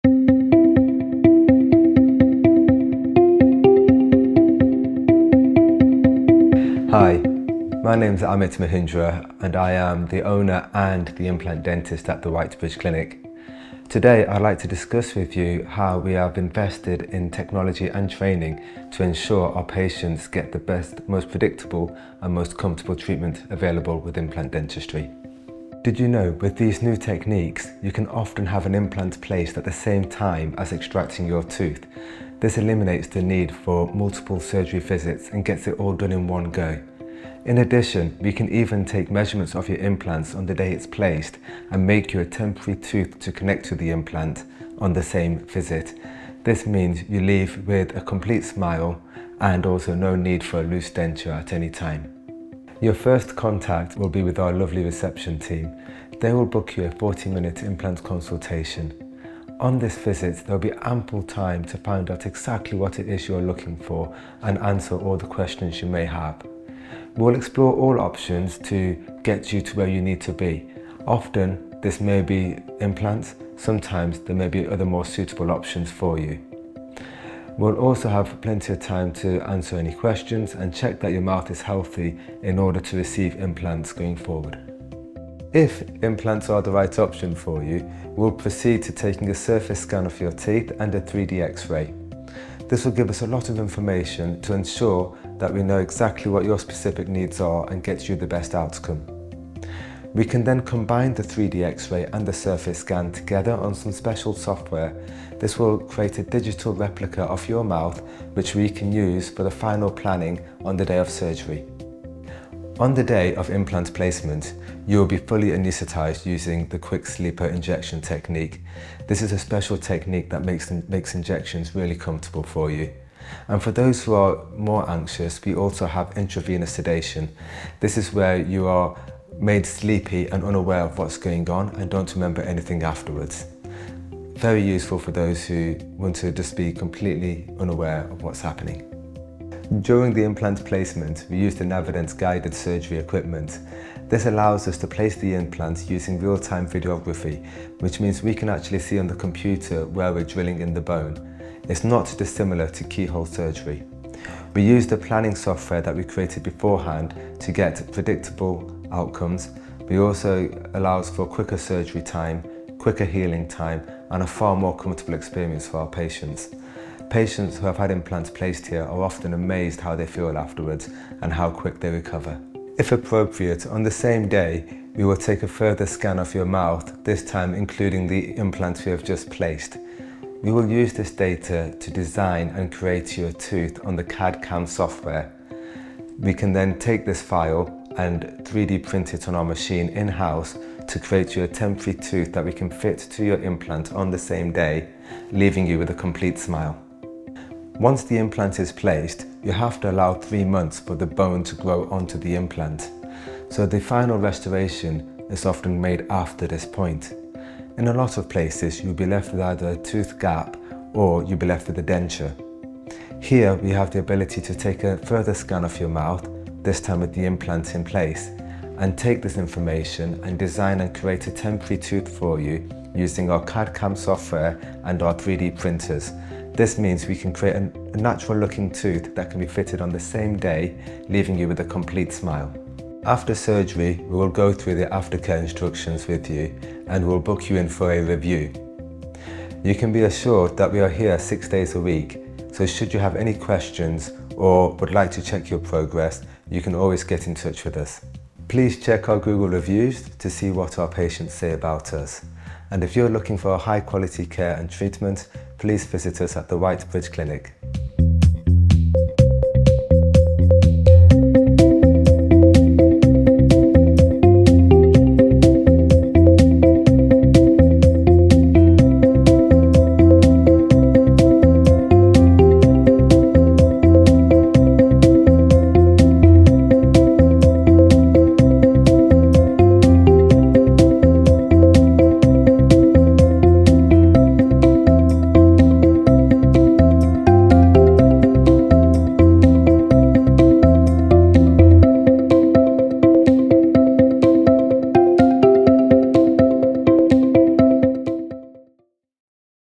The White Bridge Clinic: Quality implant and restorative dentistry with Dr Amit Mohindra
Leading implant and restorative dental surgeon, Dr Amit Mohindra explains what you can expect if you have a consultation at The White Bridge Clinic in Oxford, including how they have invested in the most cutting edge technology and training, to ensure that the care you receive is second to none.

If you would like to enquire about a consultation at The White Bridge Clinic, visit the Top Doctors profile